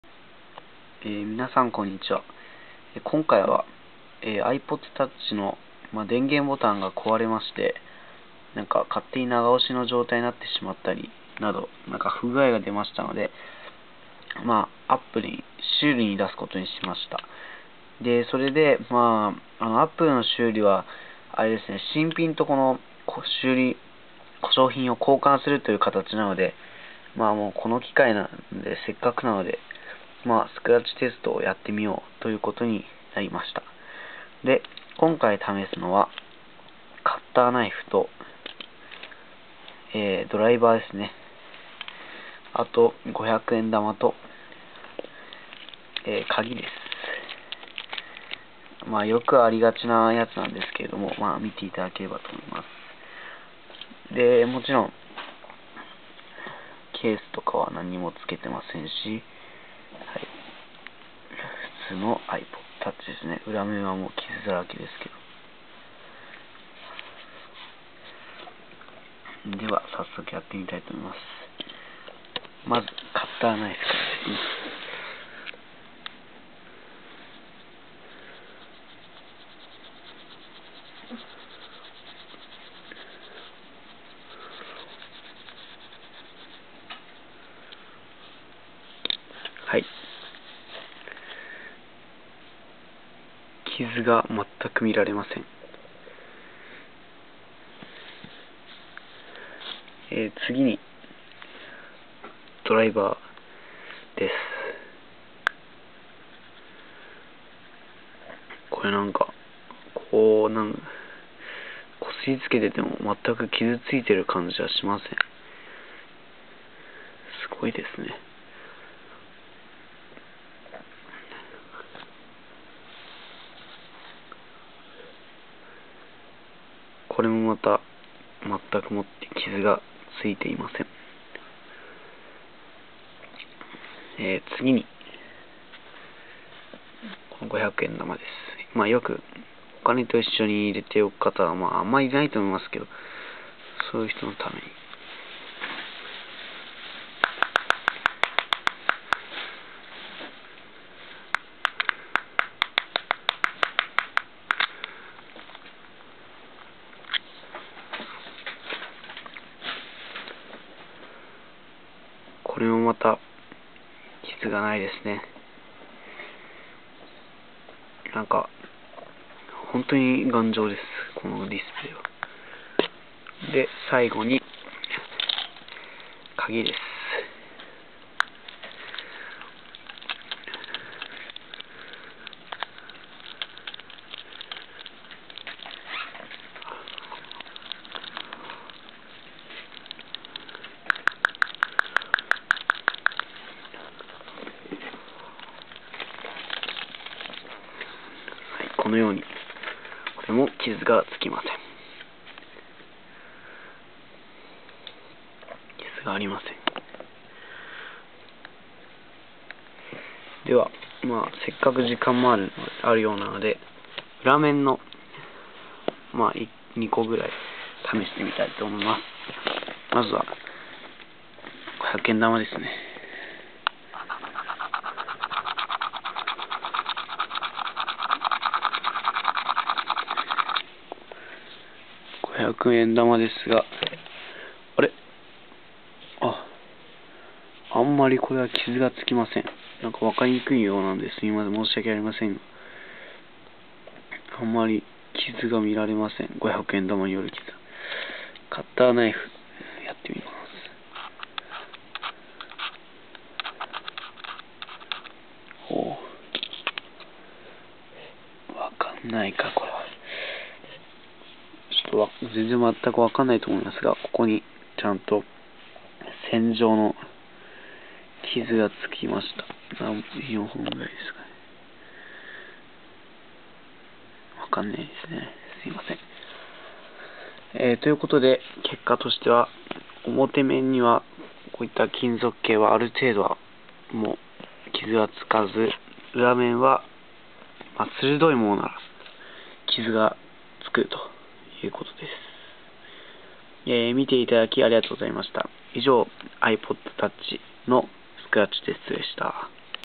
ipod touch スクラッチテスト scratch test
ipod touchが故障したので、スクラッチテストをやってみました。
いや～、それにしても頑丈です。こすっても、つるつるしてます。
皆さんも試してみてください。